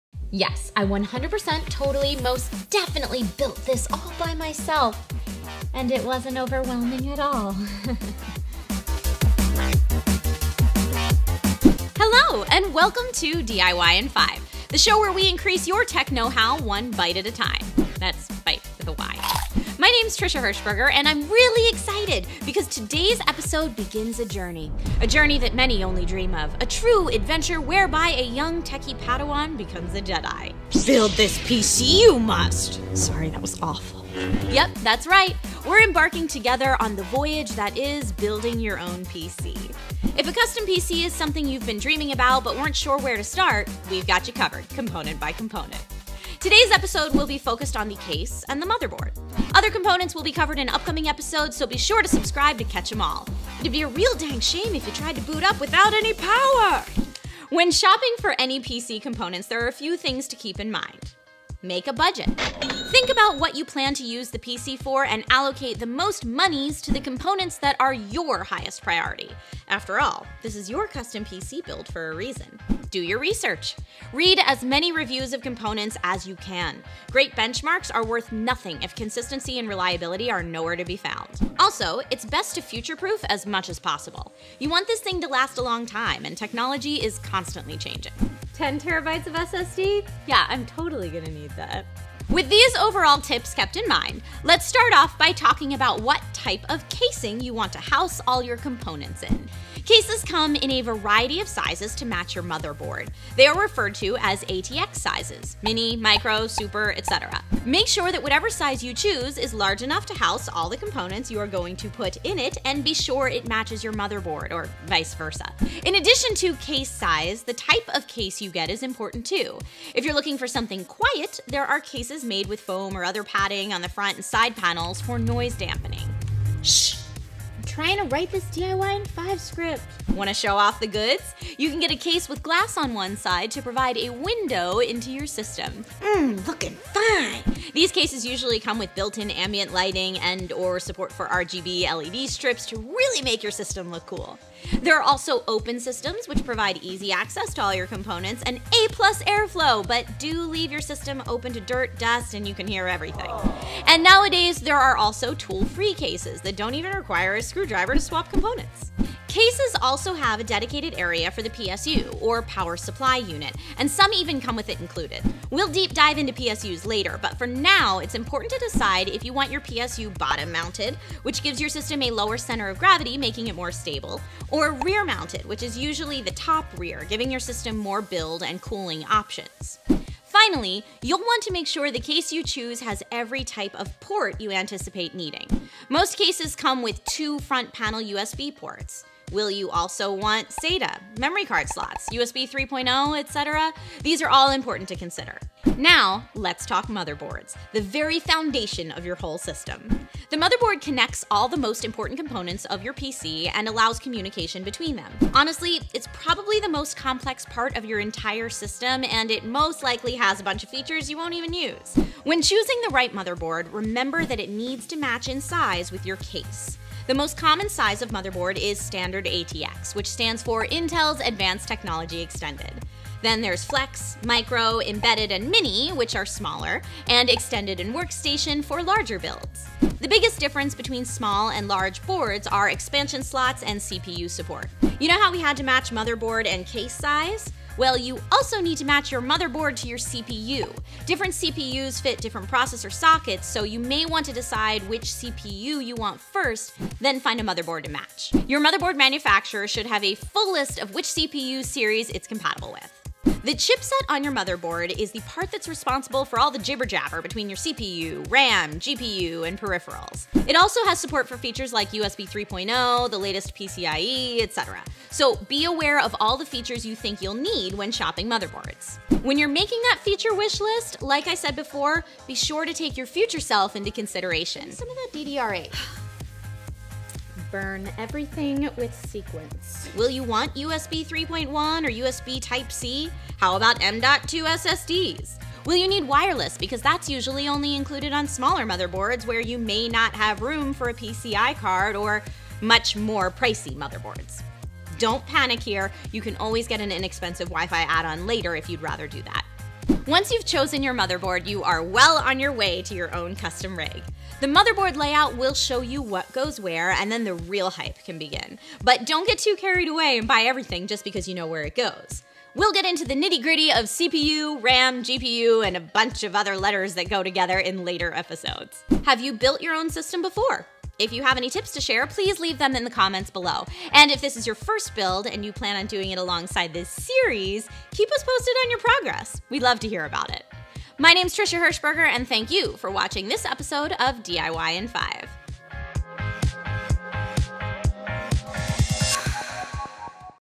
Arma tu PC - Elige el mejor Gabinete y Tarjeta Madre - DIY in 5
¡Es momento de armar tu PC! Con estos sencillos consejos, elige el mejor gabinete y tarjeta madre para tu PC. ¡Síguenos y no te pierdas de más consejos!